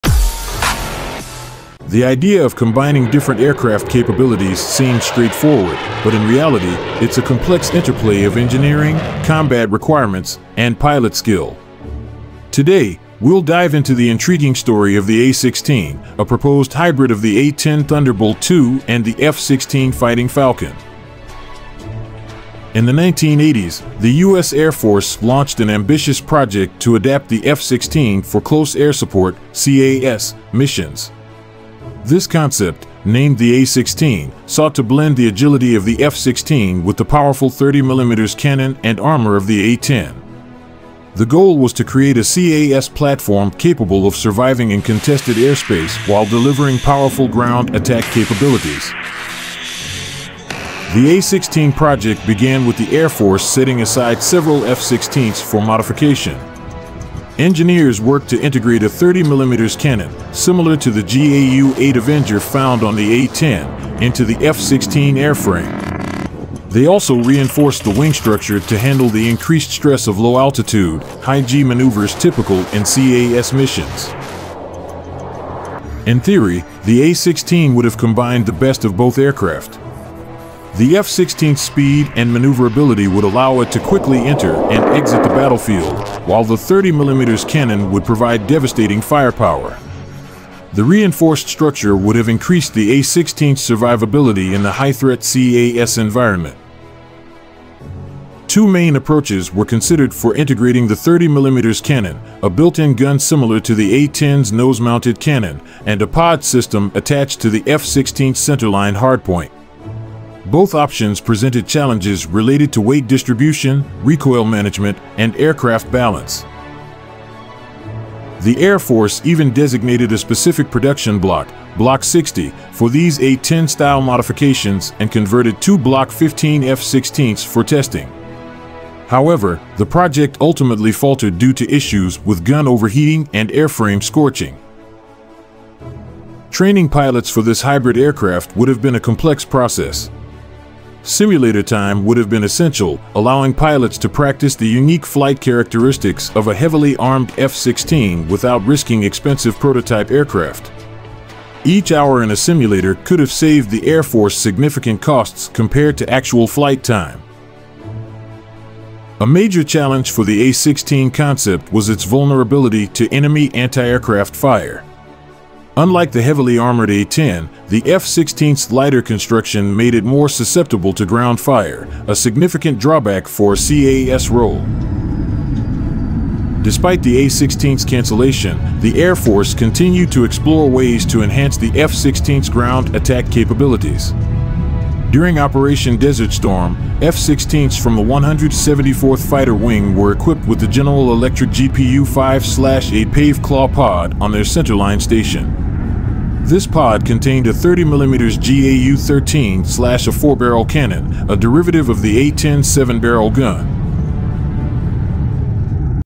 "Why Merging F-16 Speed with A-10 Firepower Was So Difficult
Explore the failed A-16 project—an attempt to combine the F-16 and A-10 into one powerful aircraft.
 Learn why the A-16 hybrid aircraft concept never made it into service.
The A-16: A bold idea that tried to blend speed and firepower but ultimately fell short.